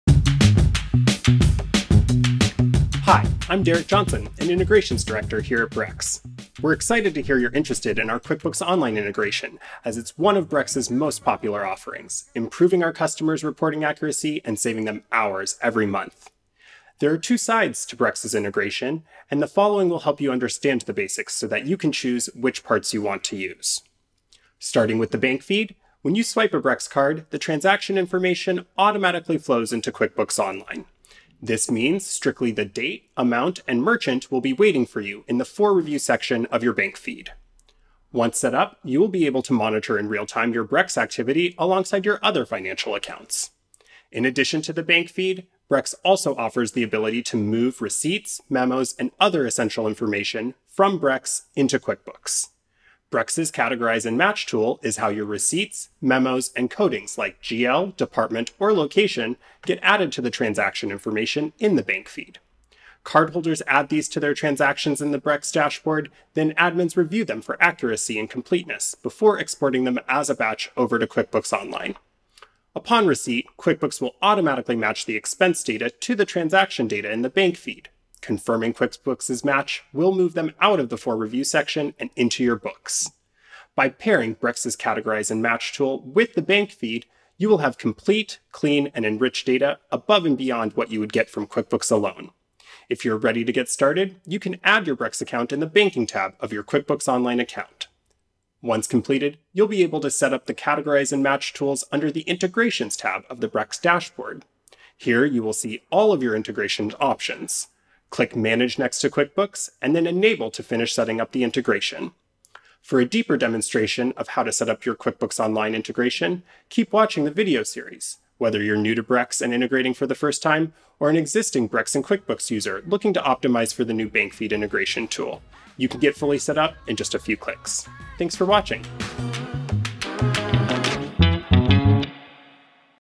QuickBooks Online integration walk-through in 2 minutes [Brex Integrations]
Brex integrates directly with QuickBooks Online, offering bank feed and categorization and receipt match, making accounting faster and more accurate than ever.